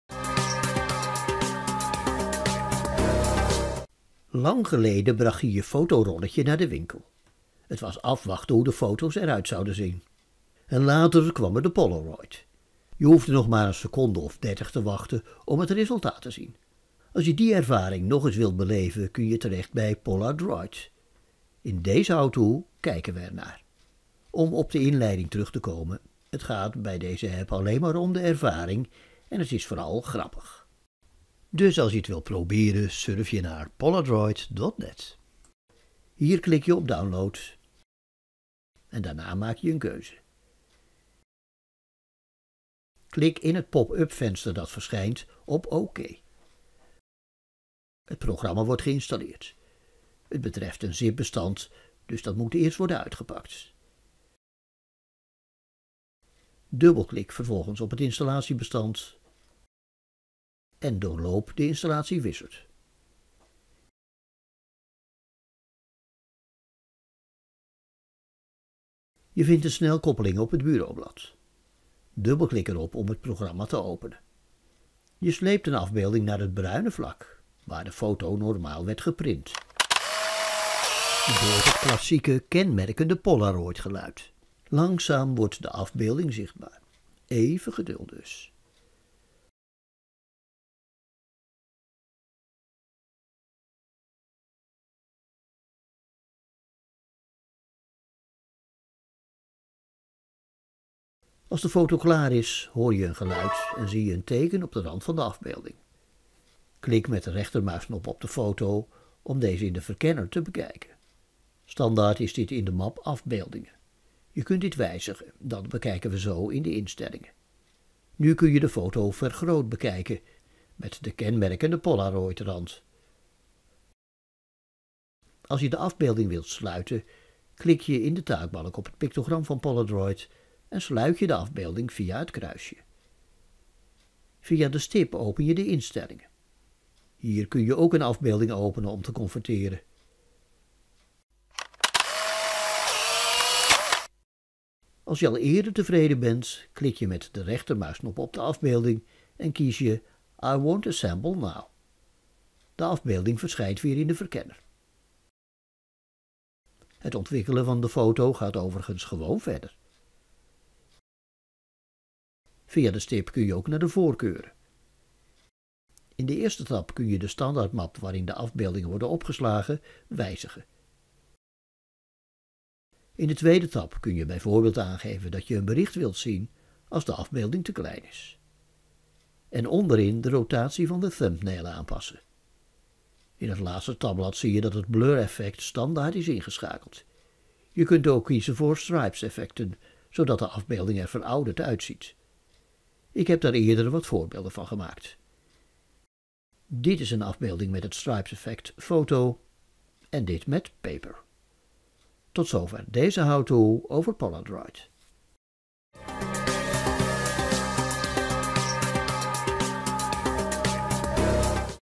Poladraid
Lang geleden bracht je je fotorolletje naar de winkel. Het was afwachten hoe de foto’s eruit zouden zijn. En later kwam er de polaroid! Je hoefde nog maar zo’n 30 seconden te wachten om het resultaat te zien. Als je die ervaring nog eens, of voor het eerst, wilt beleven, kun je terecht bij Poladroid.